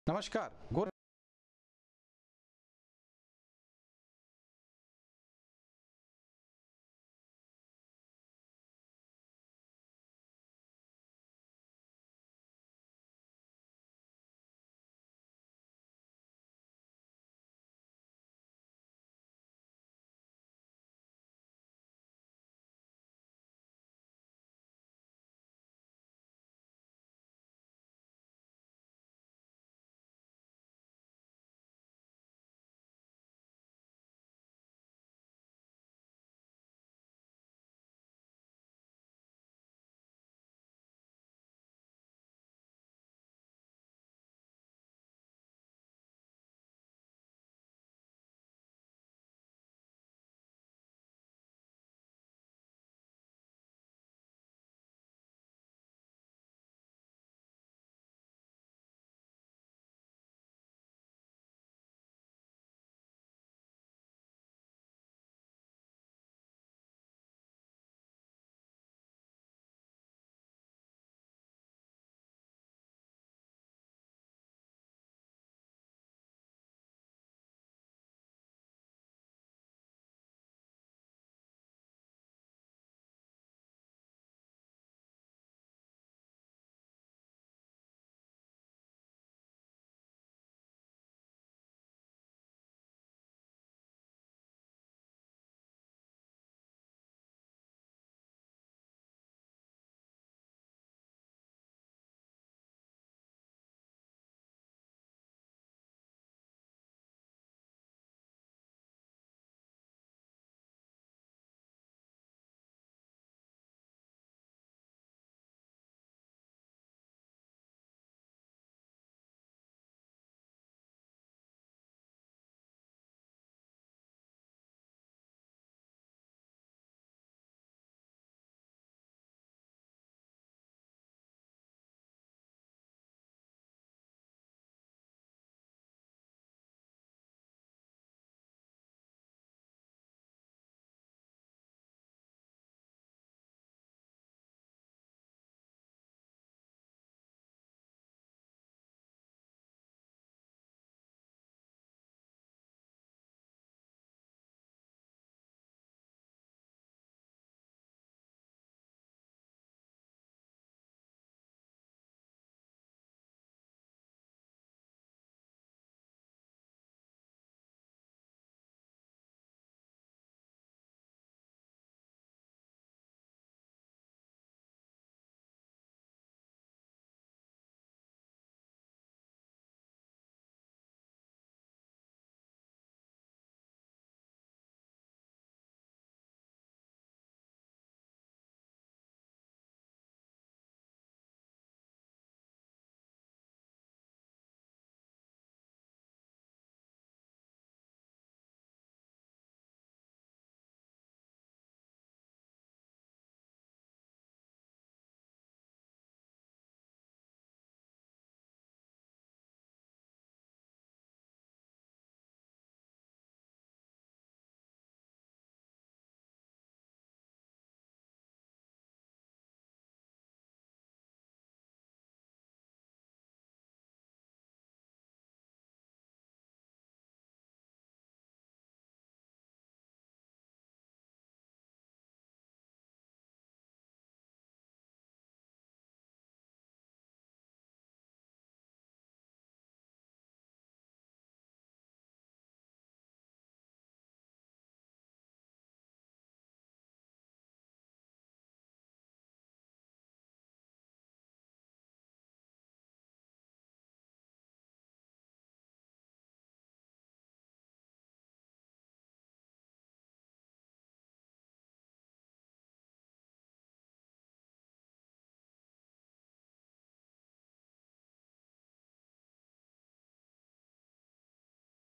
गोरखपुर पुलिस की टॉप फाइव माफिया के लिस्ट में शामिल विनोद उपाध्याय के मुग़लहा स्थित अवैध मकान
गोरखपुर पुलिस की टॉप फाइव माफिया के लिस्ट में शामिल विनोद उपाध्याय के मुग़लहा स्थित अवैध मकान पर चला प्रशासन का बुल्डोजर,पचास हजार का इनाम है घोषित,चल रहा है वांछित फरार ,एसपी सीटी ने कहा जल्द होगी गिरफ्तारी